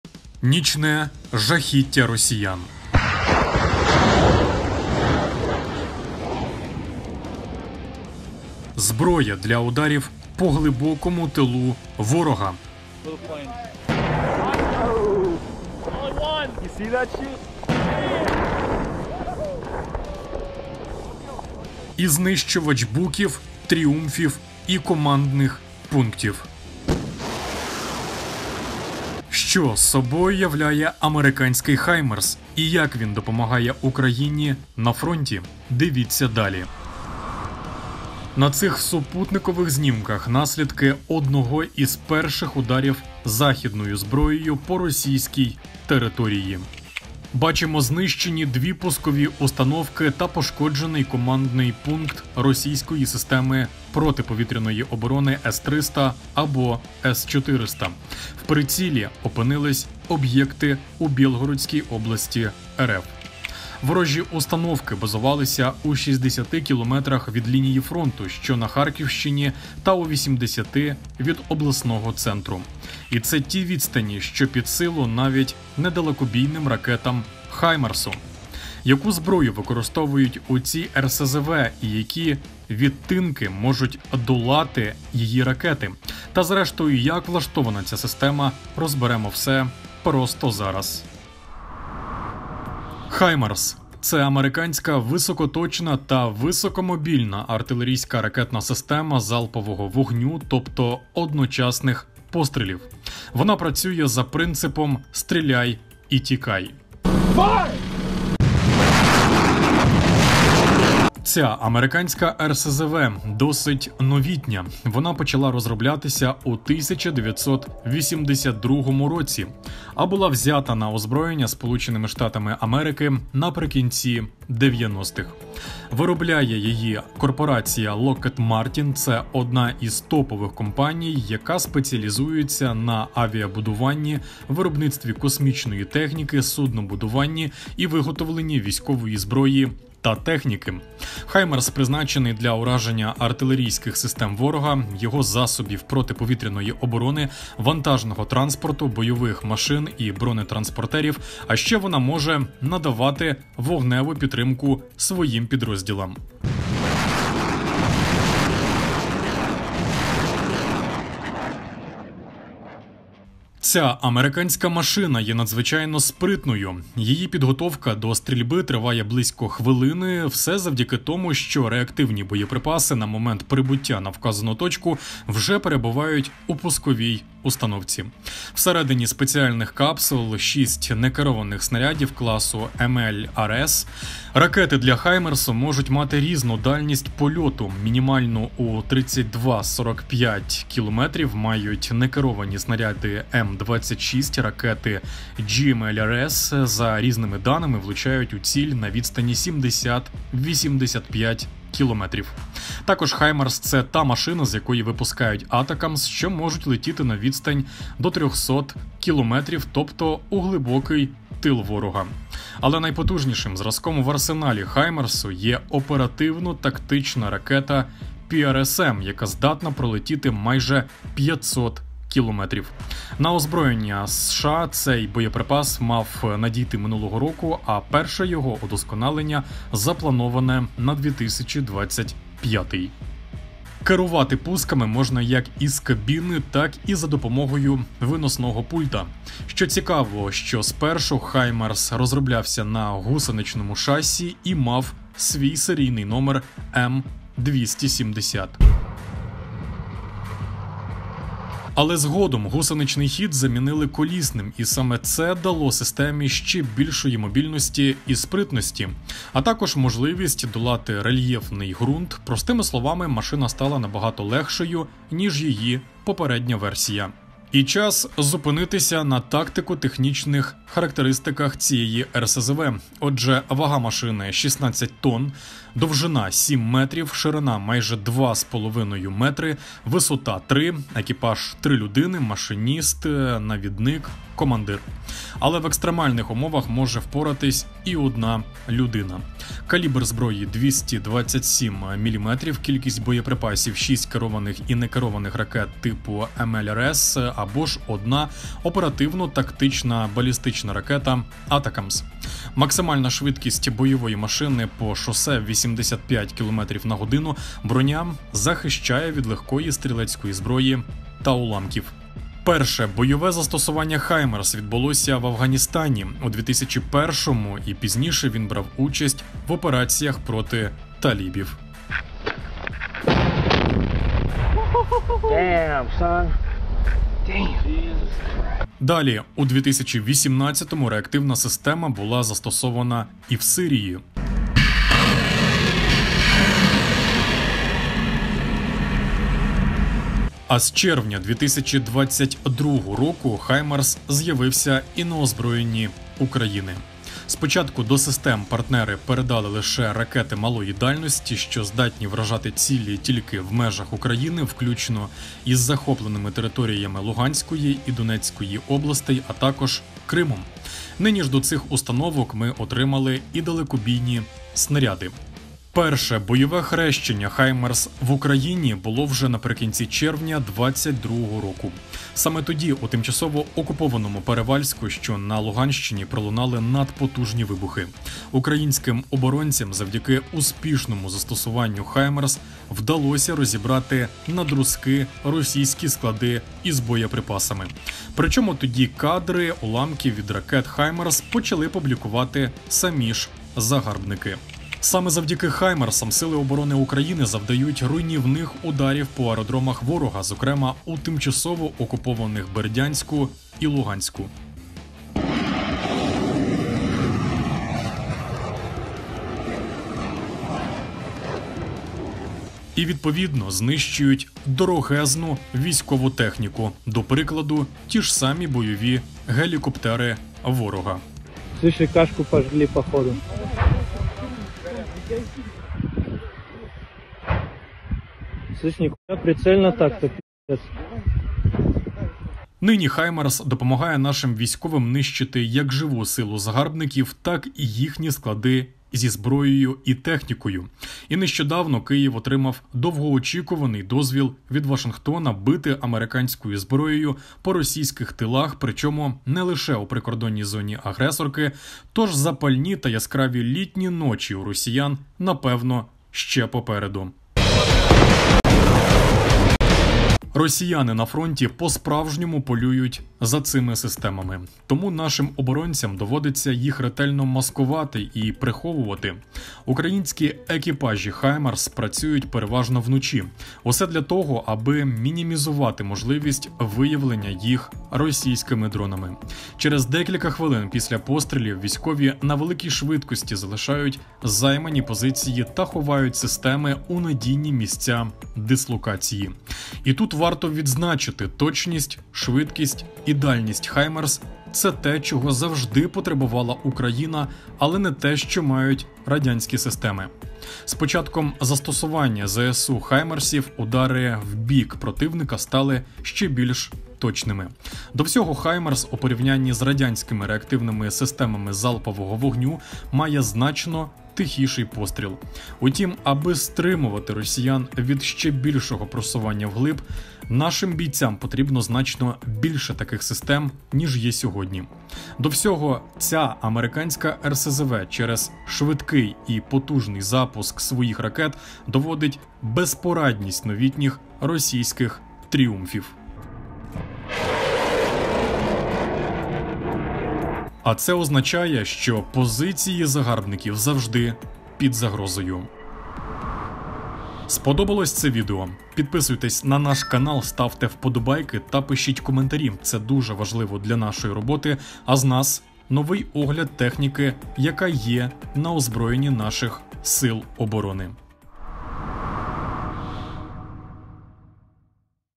HIMARS 🔥 Нічний ЖАХ росіян та ЗНИЩУВАЧ ТИЛУ ворога — ОГЛЯД
Що собою являє американський Himars? І як він допомагає Україні на фронті? Про нічне жахіття росіян, зброю для ударів по глибокому тилу ворога і знищувач Буків, Тріумфів та командних пунктів — дивись детальний огляд на американський Himars у новому випуску OBOZ.TECH!

Підтримайте відео вподобайкою!